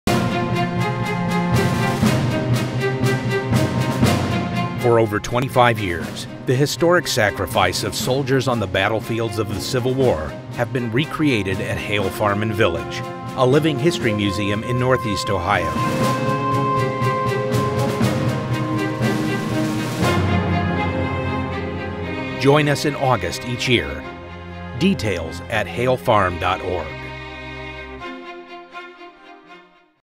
Hale Farm & Village Civil War Reenactment 2016
Join us for the annual Civil War reenactment at Hale Farm & Village on August 13th and 14th, 2016.